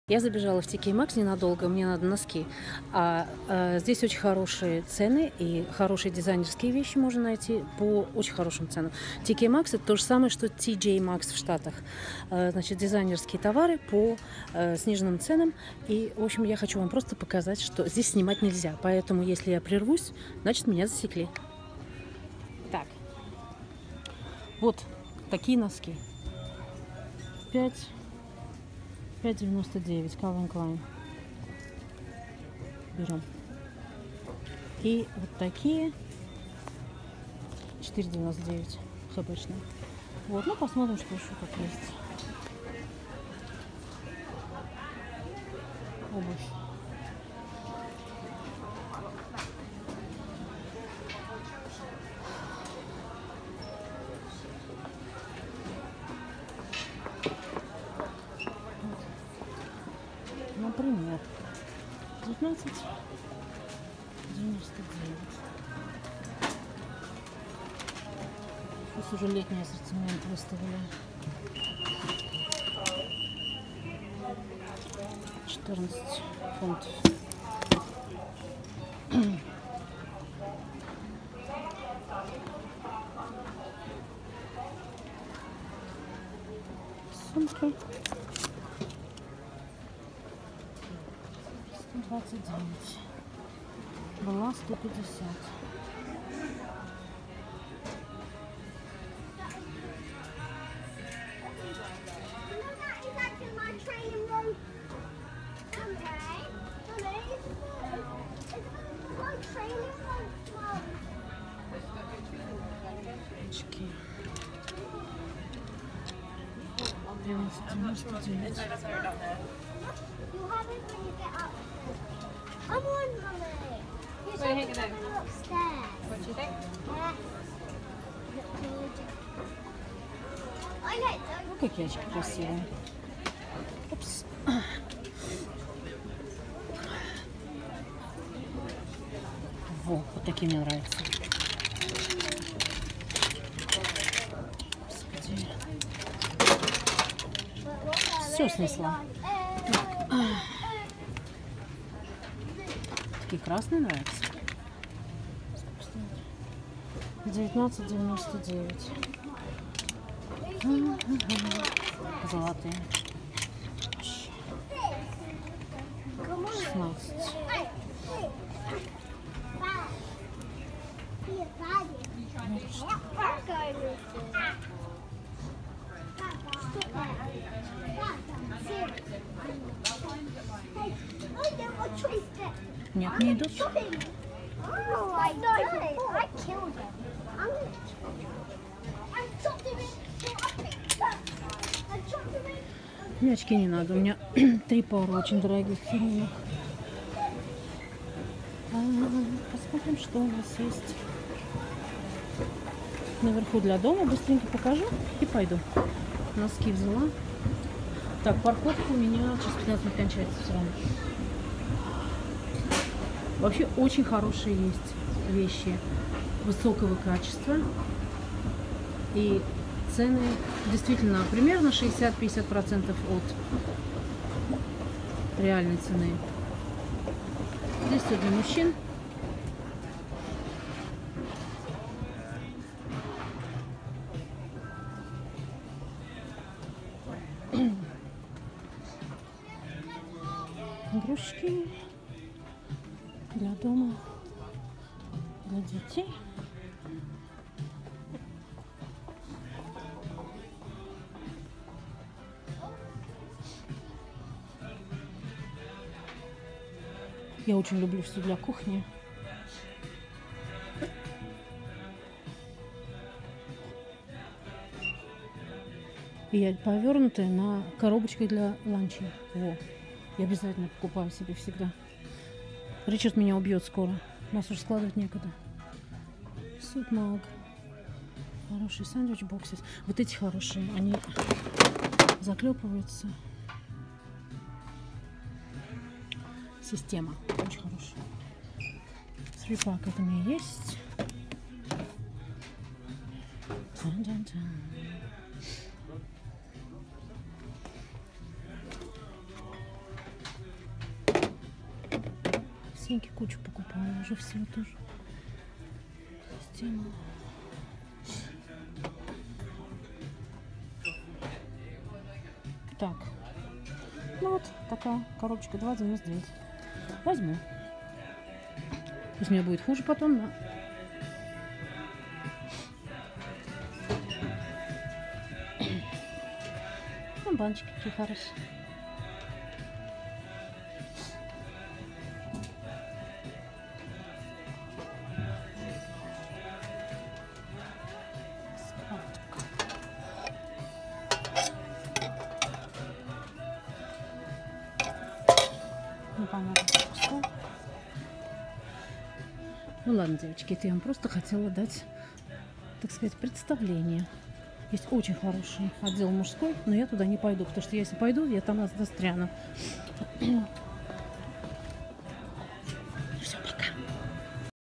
Жизнь в Англии: ТK Макс Милтон Кейнс TK MAXX в городе Milton Keynes
Быстрый пробег по ТК МАКСу